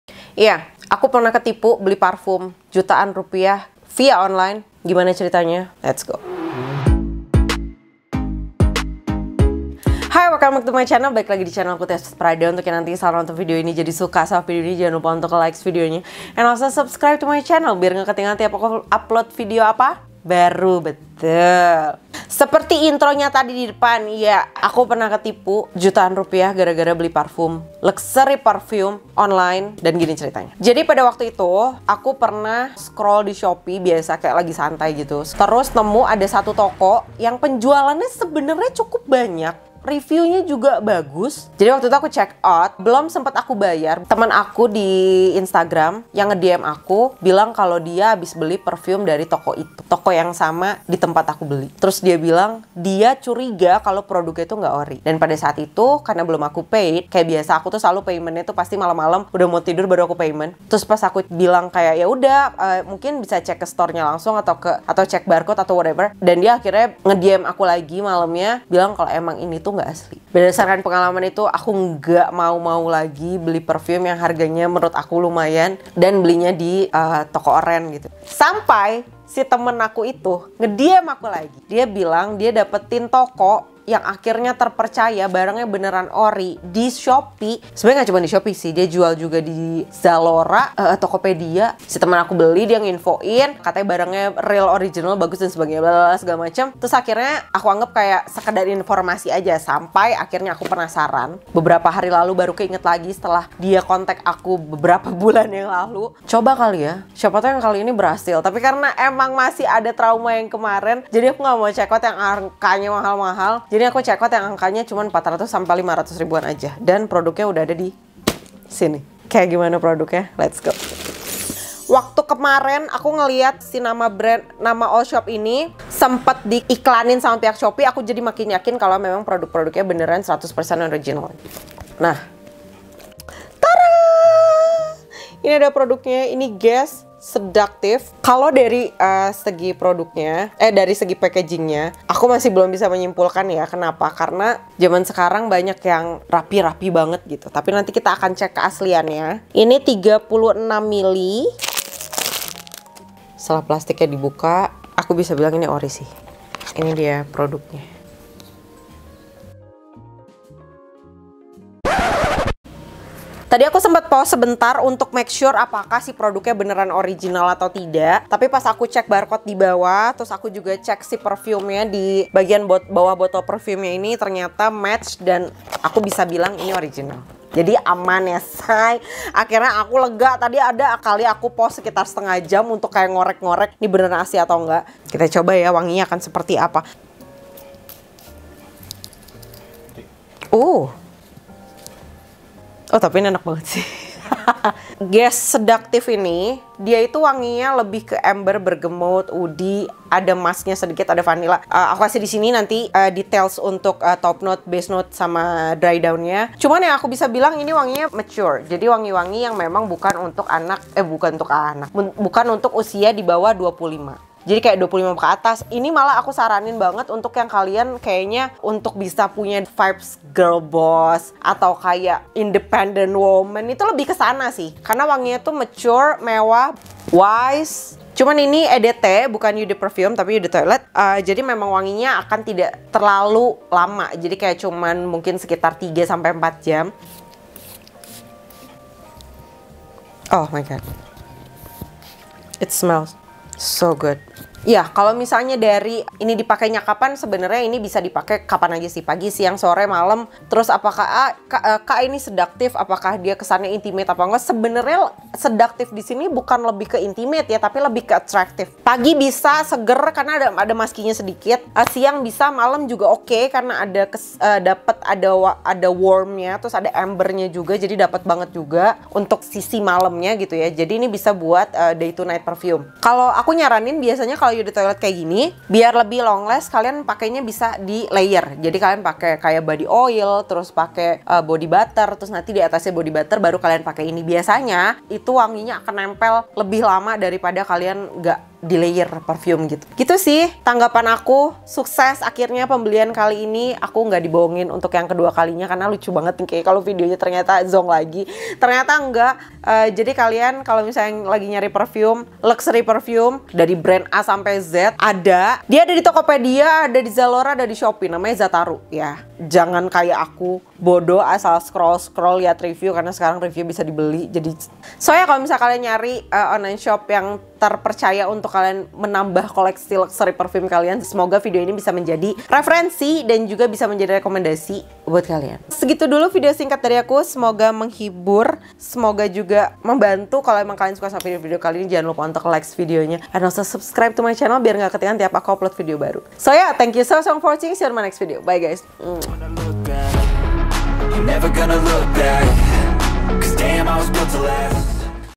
AKHIRNYA NEMU OLSHOP BUAT BELI PARFUM BRANDED YANG BENERAN ORIGINAL ! HAUL SHOPEE PARFUM BRANDED ORI
hai haiiiii... kali ini aku sharing tentang pengalaman aku nih yang ketipu pas mau beli parfum luxury di shopee, tapiiiiiii berakhir bahagia soalnya akhirnya aku nemuin toko online yang beneran ori dan terpercaya banget nih. koleksinya juga cukup lengkap sih menurut aku. cerita ketipunya gimana? nonton sampai selesai yaaa.

biar kamu ga ketipu juga link toko yang jual nya beneran ori aku taro disini ya:

timestamp :
0:00 -  intro
0:21 - cerita ketipu beli parfum mahal di shope
1:18 - beli parfum branded di shopee
2:03 - unboxing parfum branded beli di shopee
3:08 - review parfum guess seductive
4:52 - zataru
5:15 - closing

DILARANG KERAS MENGGUNAKAN, RE-UPLOAD, MEMPUBLISH VIDEO INI UNTUK KEPENTINGAN PRIBADI DENGAN TUJUAN DAN ALASAN APAPUN